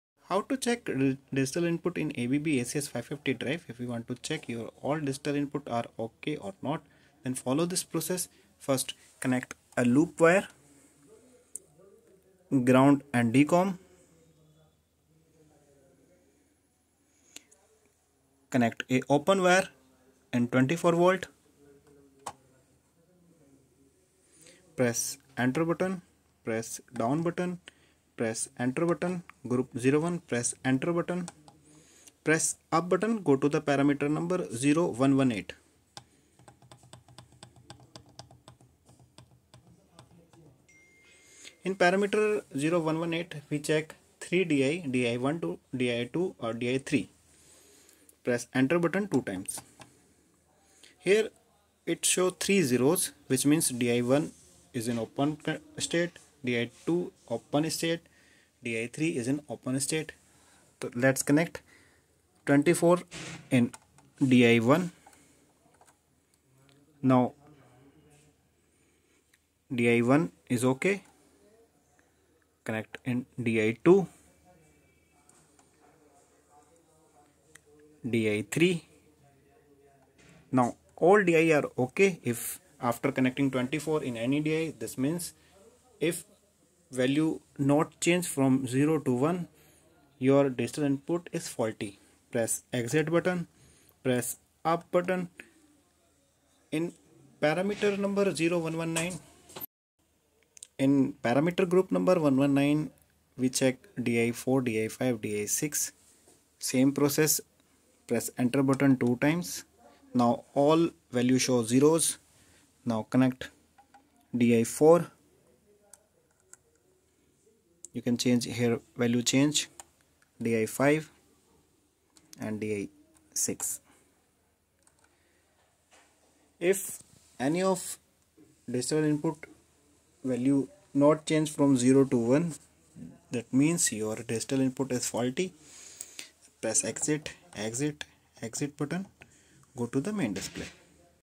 How to check digital input DI in ABB ACS550 drive?
Learn how to effectively monitor and troubleshoot your ABB ACS550 drive by checking digital input (DI) status. This guide provides clear steps on accessing and interpreting DI information, helping you diagnose issues and optimize your system's performance.